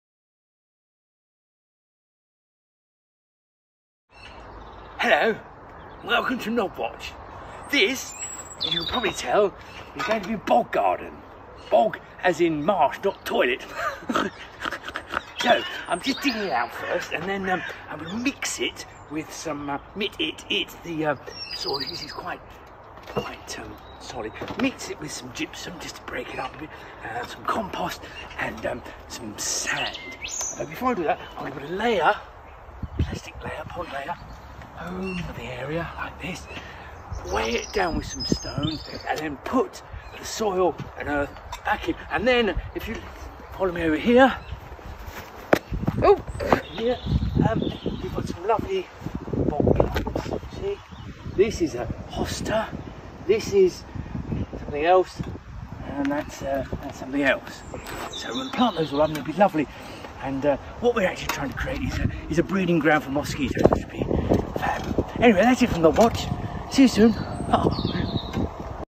Nob watch on the bog
Nob watch shows how to prepare the perfect habitat for moisture loving plants